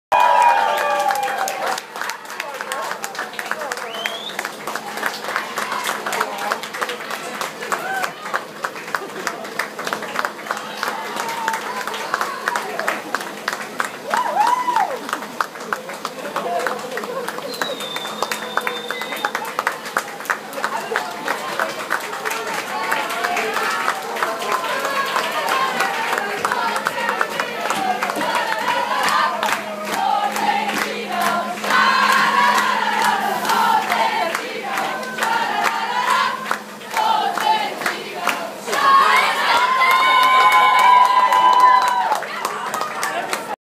Frauenfussball-EM Nadine Angerer
German woman soccer team arriving at the hotel with the EM trophy 2013